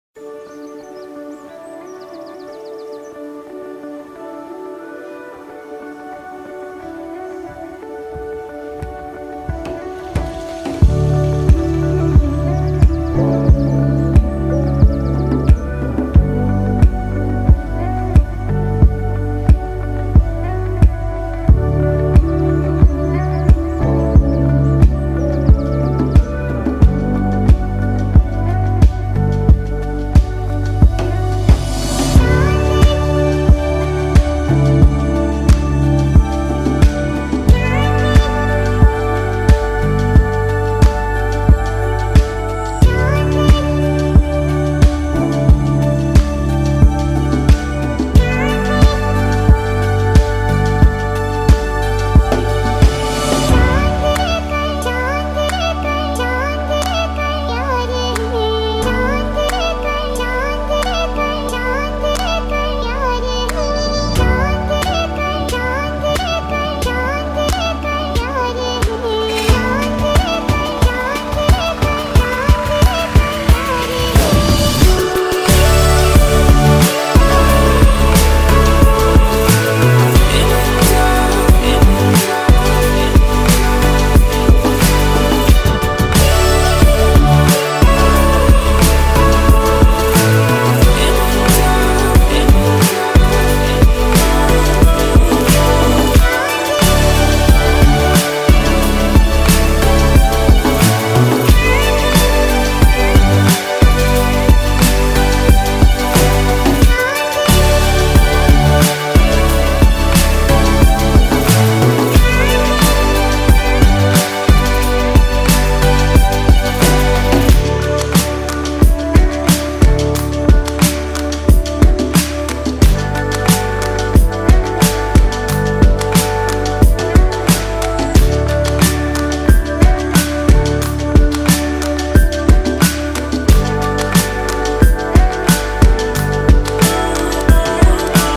Rishikesh, Haridwar India | DJI Spark | iPhone X
Shots from Rishikesh and Haridwar
Using DJI Spark and iPhone X